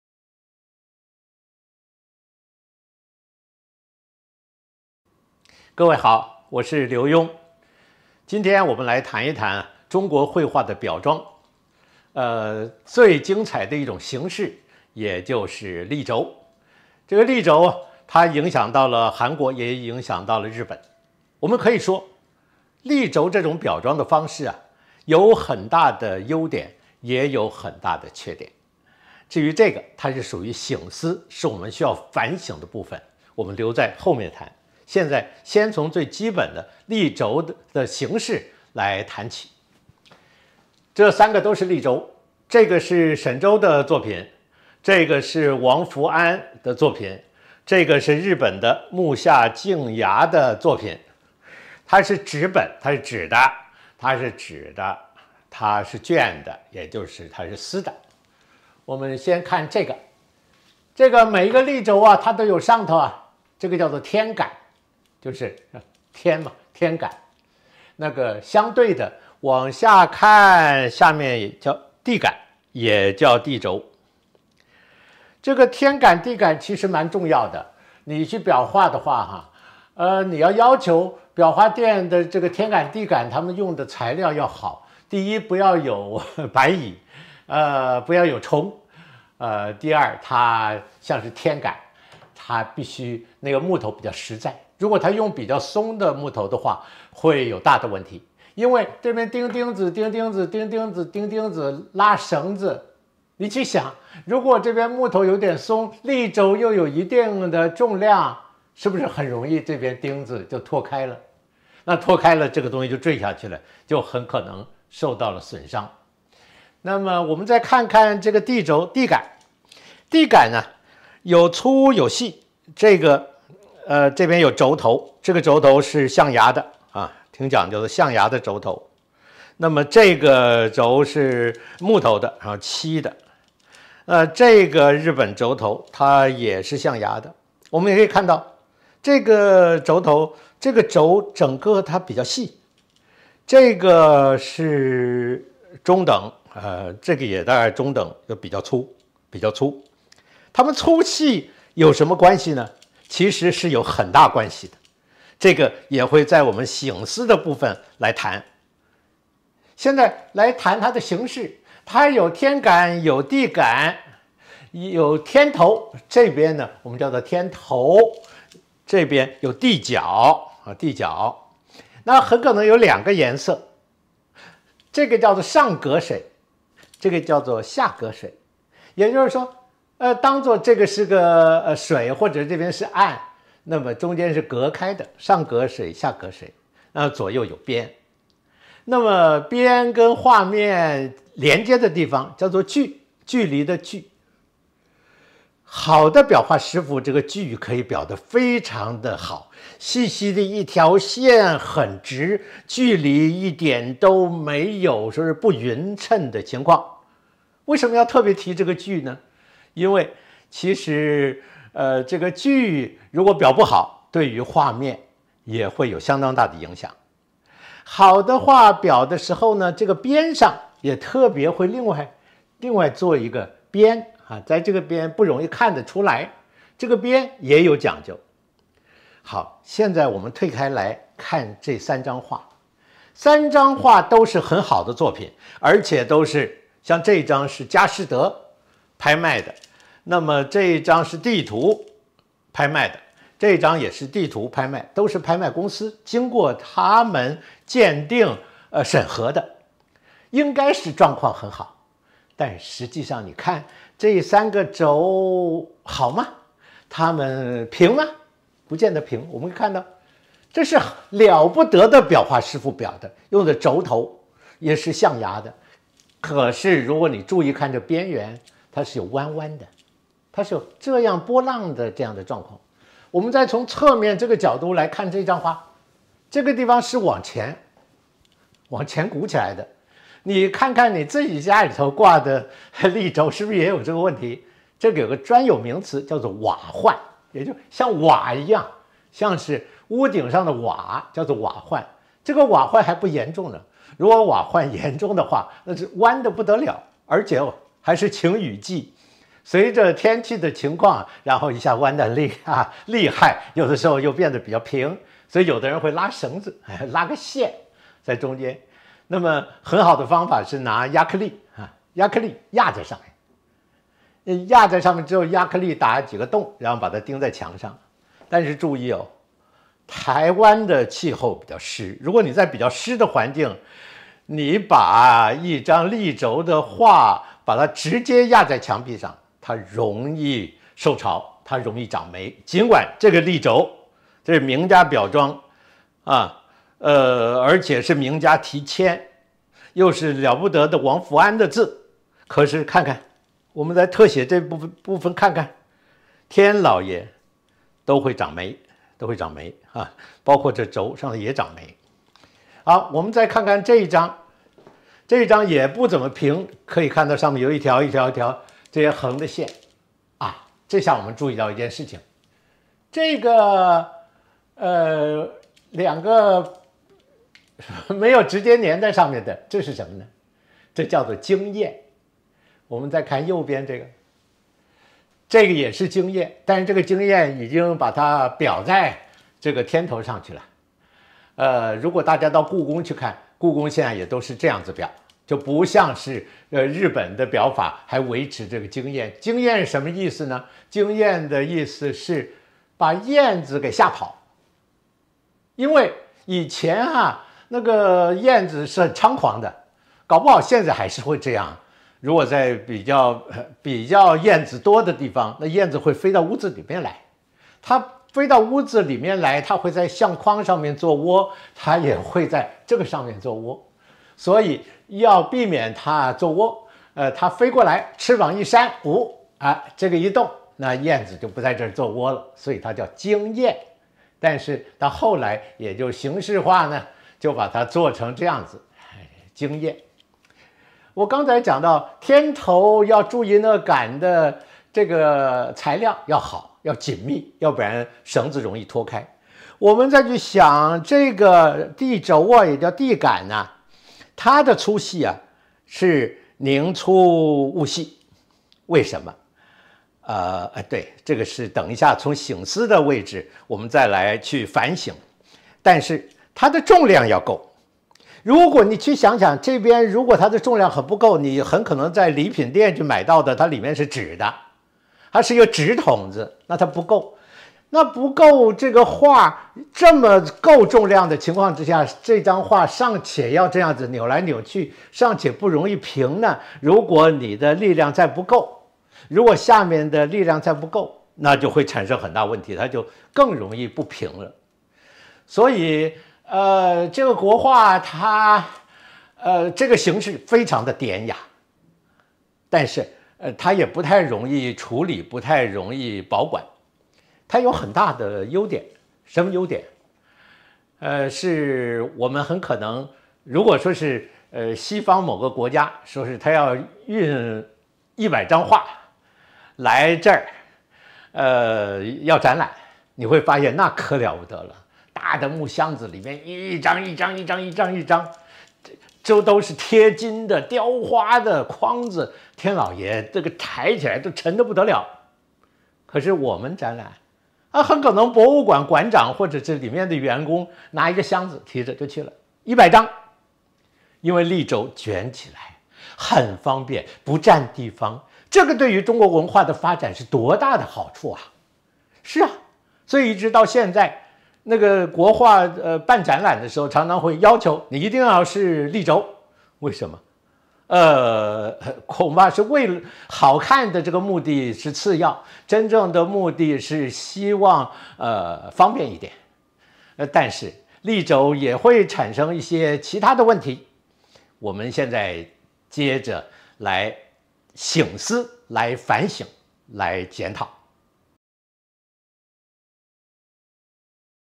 劉墉講堂──中國繪畫的省思1
各位家裡懸掛立軸嗎？它方便不方便？簡單不簡單？但是風一吹會不會搖來擺去？外面沒有玻璃保護會不會容易髒？還有，它們會不會起皺變形？立軸對中華文化的發展有很大的貢獻，甚至影響到日本韓國。但是這種裱裝會不會也有缺點？今天讓我們一起來省思。